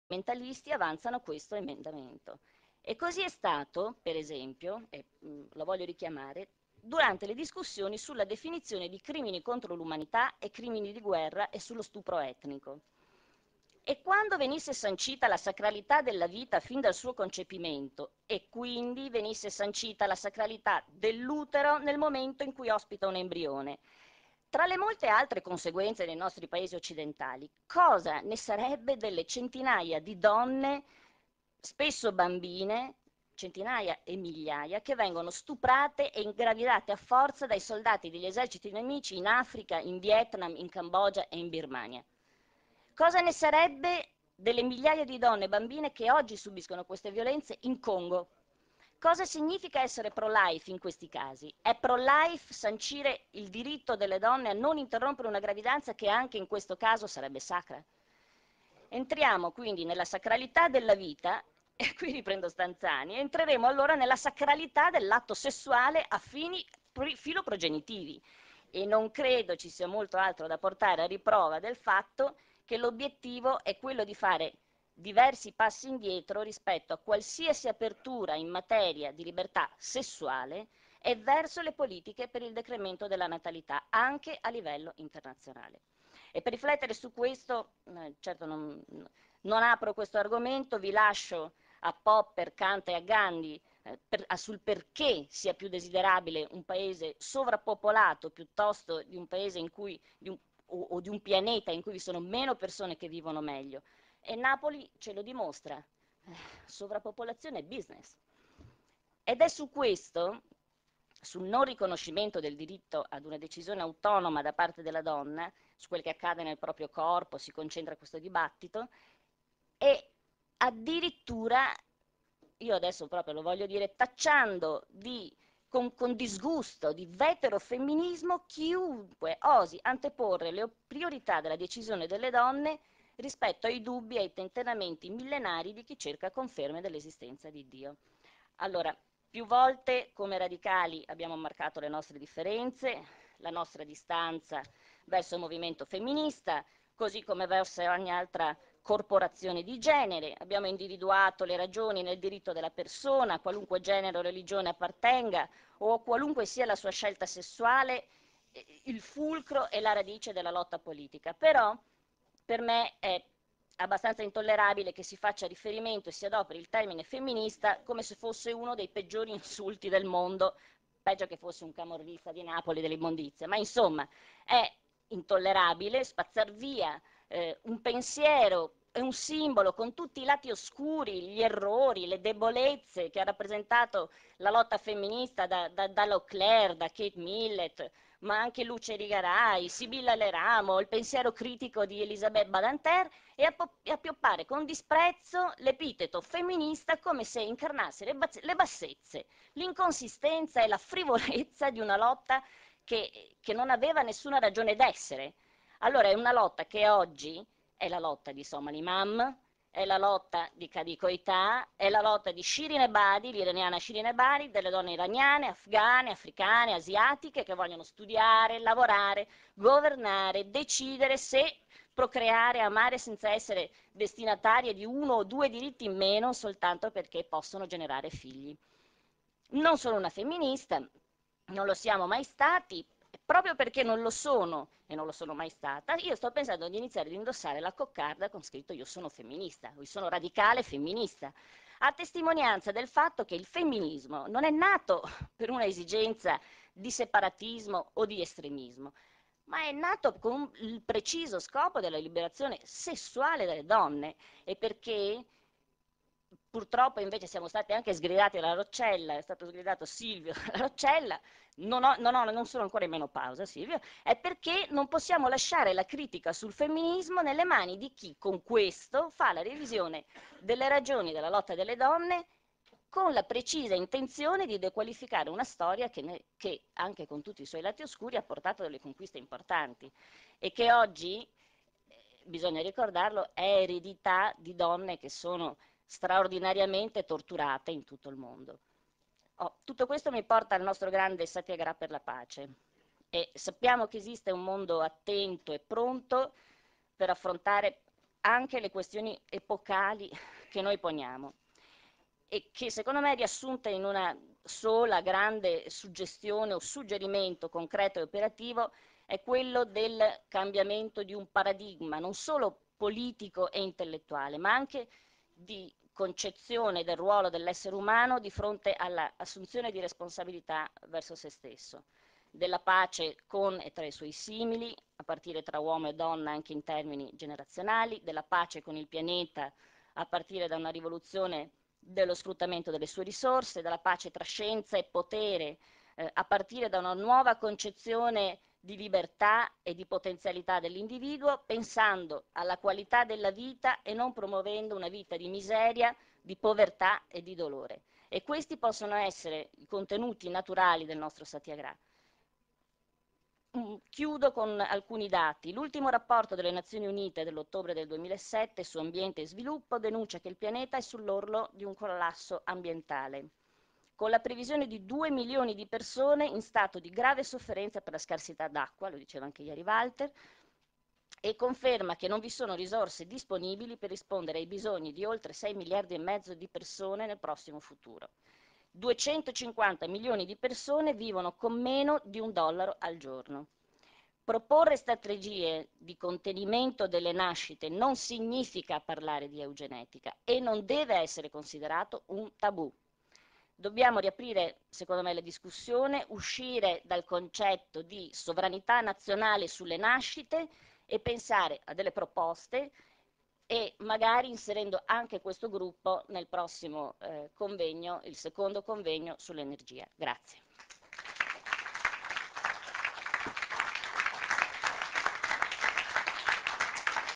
Antonella Spolaor parla della sovrappopolazione (parte 2)
Intervento di Antonella Spolaor dell'Associazione Rientrodolce al Comitato Nazionale di Radicali Italiani (12/01/2008)